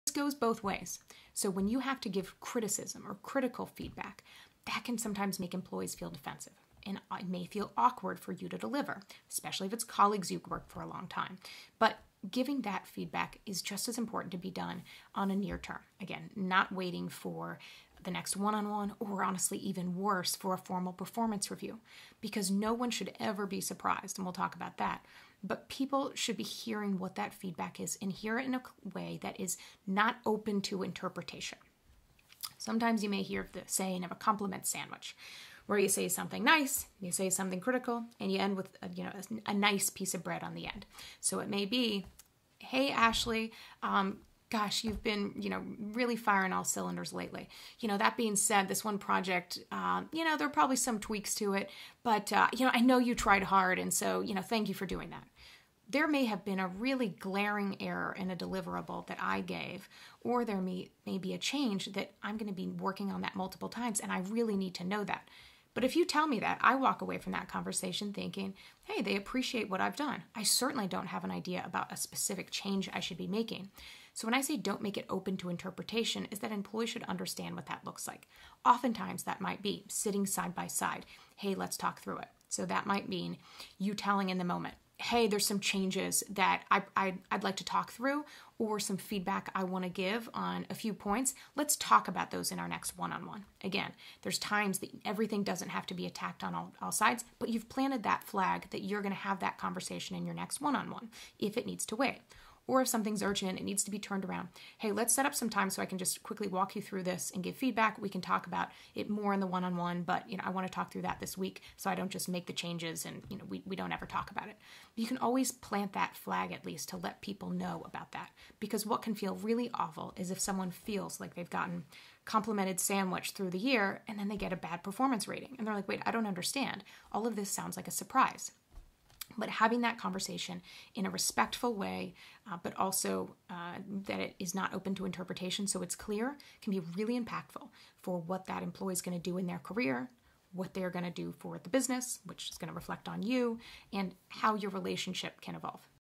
Manager Method Training - Content Example - Critical Feedback
Example of section of Manager Manager Training Modules content (performance management)